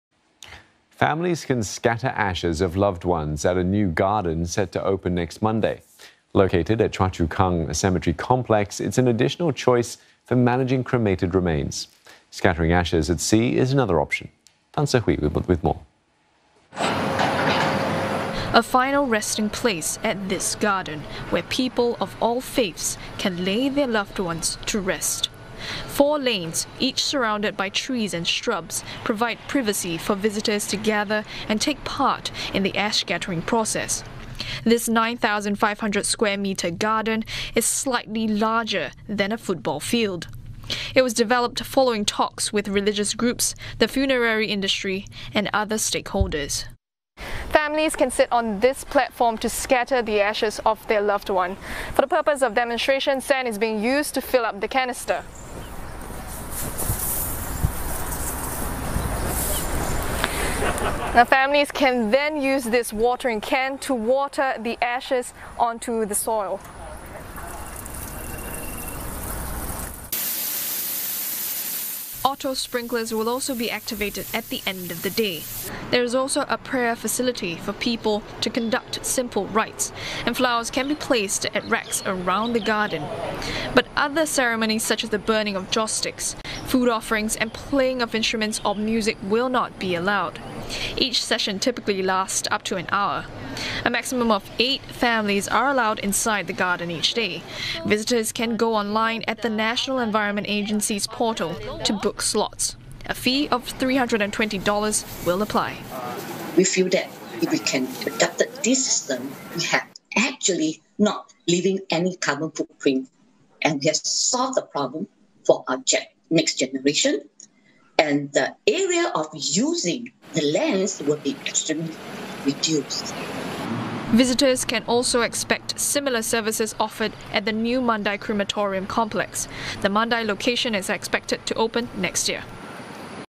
GARDEN OF PEACE @ CCK Inland ash scattering garden at Choa Chu Kang Cemetery to open on May 17
GARDEN OF PEACE @ CCK CNA NEWS  Families can scatter ashes of loved ones at a new garden at Choa Chu Kang Cemetery Complex. Set to open on May 17, it's an additional choice for managing cremated remains. Four lanes, each surrounded by trees and shrubs, provide privacy for visitors to take part in the ash scattering process at the 9,500 sqm garden. Auto sprinklers will be activated at the end of the day. There's also a prayer facility for people to conduct simple rites and flowers can be placed at racks around the garden. But the burning of joss sticks, food offerings and playing of instruments or music will not be allowed.